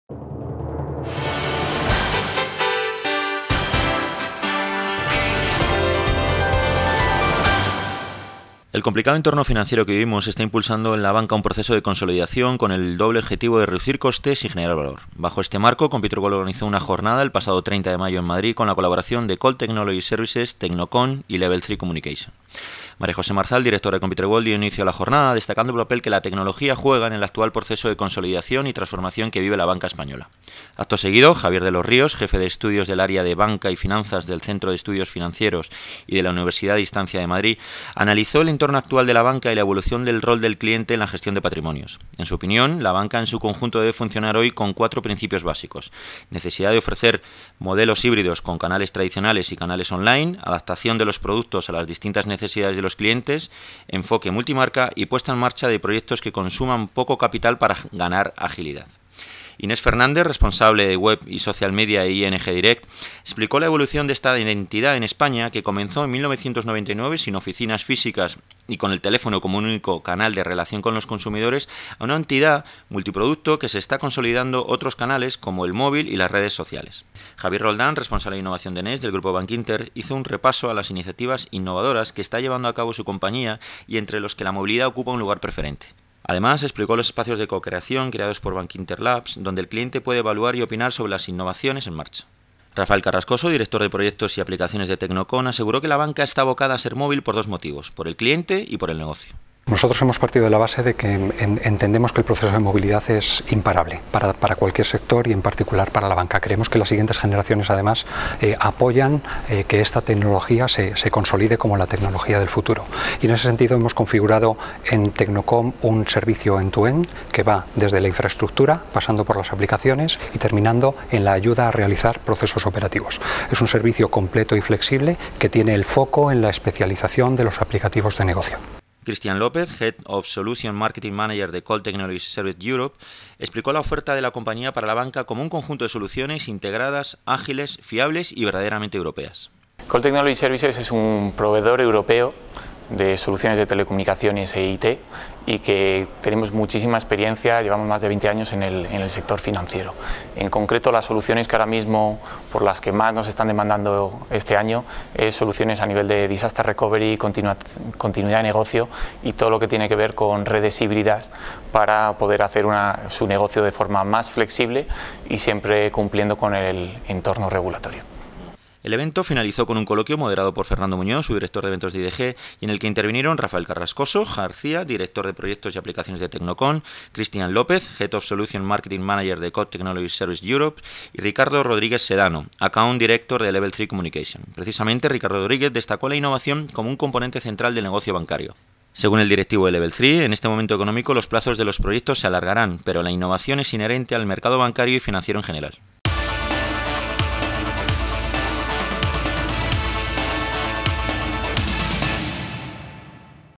La banca del futuro
El complicado entorno financiero que vivimos está impulsando en la banca un proceso de consolidación con el doble objetivo de reducir costes y generar valor. Bajo este marco, ComputerWorld organizó una jornada el pasado día 30 de mayo en Madrid con la colaboración de Colt Technology Services , Tecnocom y de Level 3 Communications.